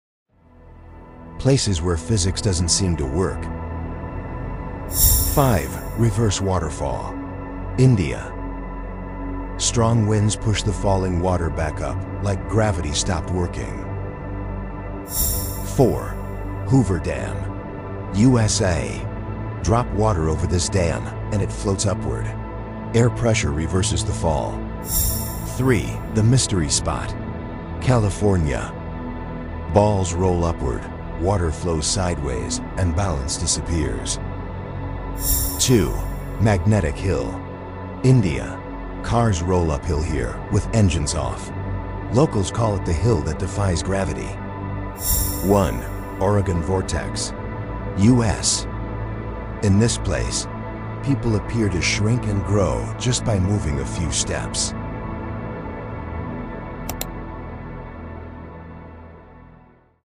Places Where Physics Doesn't Seem To Work
🌍 These are 5 real places on Earth where physics seems to stop working.
From gravity-defying hills to waterfalls that rise instead of falling.

🎬 Watch till the end and see which one confuses you the most!